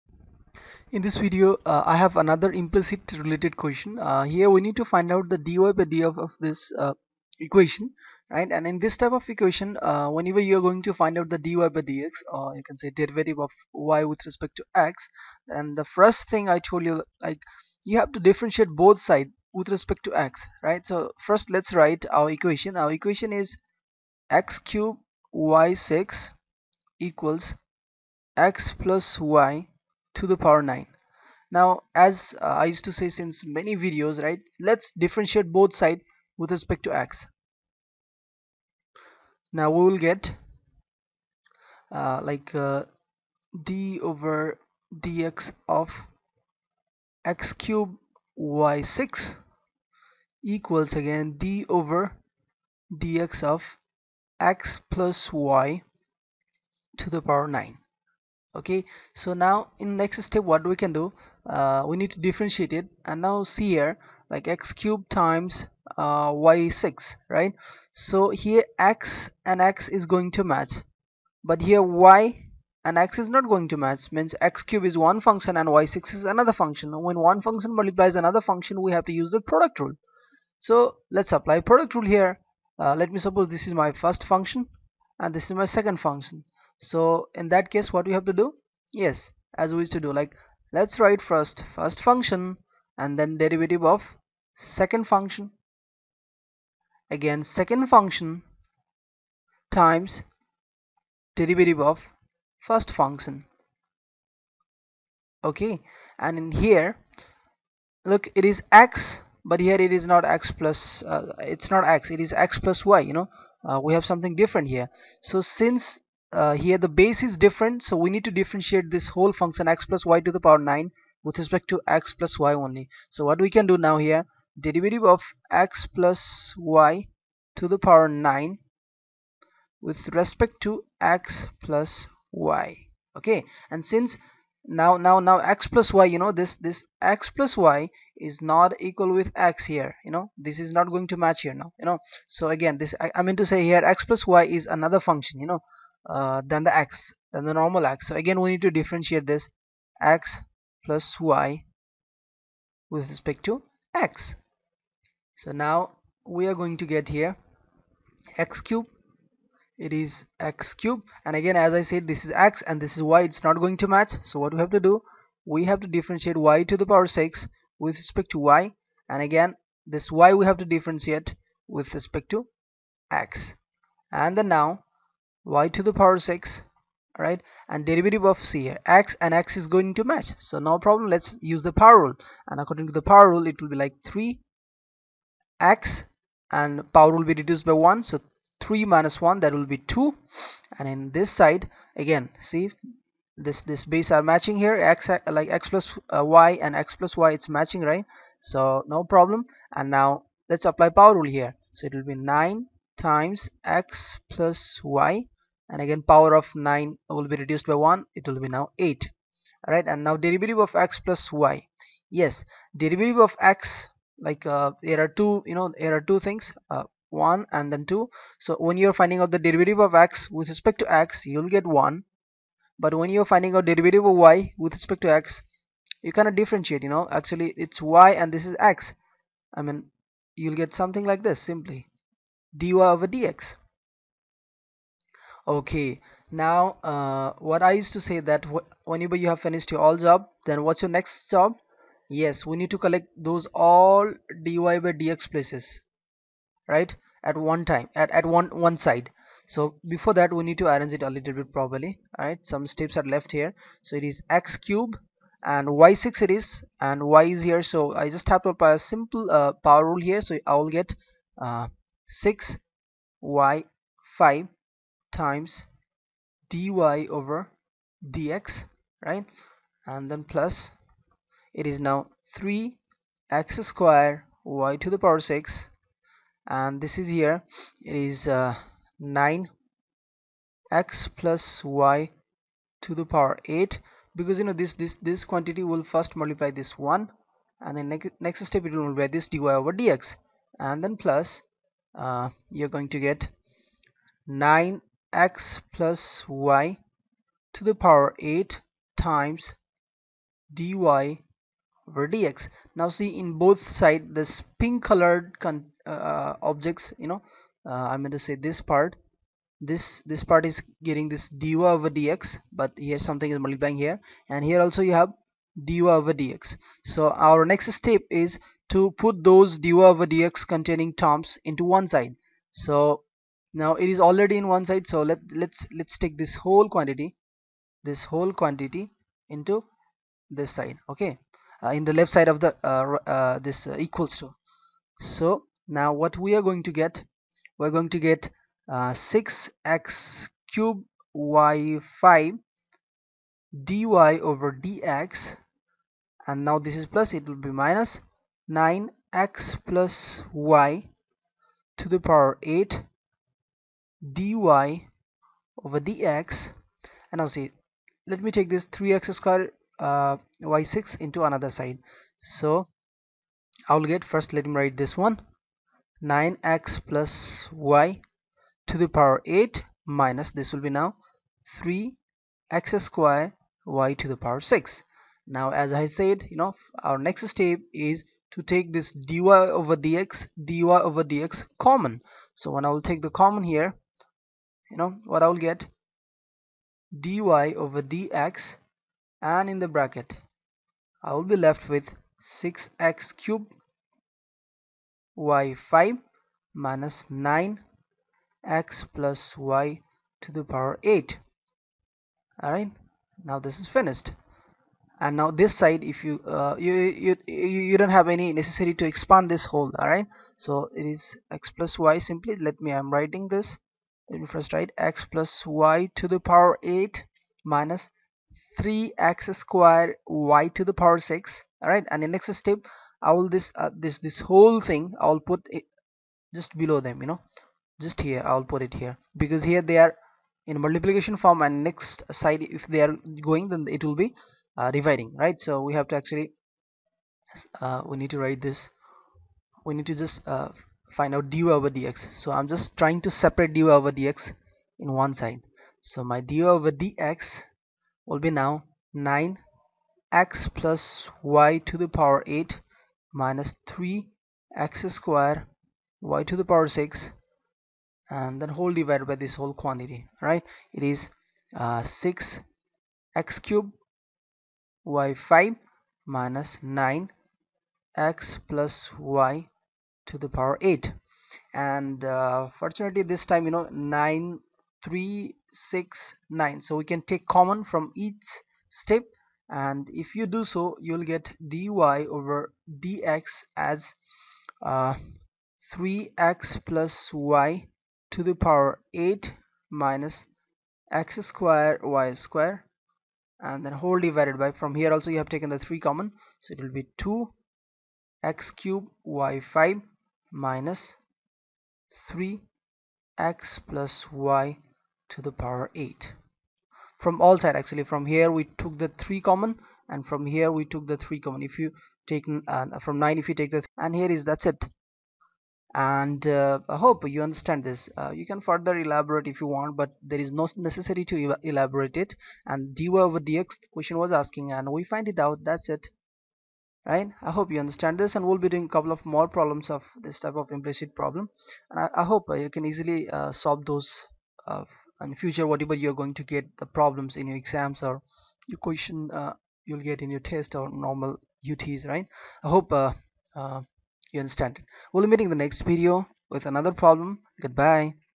Implicit Differentiation 7
For Complete List of Tutorials on Derivative Calculus.

This is 7th tutorial for Implicit Differentiation - . In this video I have find the derivative of a implicit function.  Derivative of a Implicit function simply means finding derivative of a implicit function. Implicit function are something like mixed type of function both in the in the form of X & y mixed like X^2 + y^2  + 9 xy = 45 etc. And the process of finding derivative of such implicit function is called implicit differentiation.
To Find the derivative of such implicit function following 2 steps we use:
1. First Write The given Equation as given in the question.
2. Differentiate both sides with respect to X
3. Collect all dy/dx term in left hand side & send other terms without dy/dx into right hand sides.
4. Take dy/dx common from all terms
5. Send all other terms into another side leaving only dy/dx in left hand sides
6. your derivative of the given implicit function is in right hand side.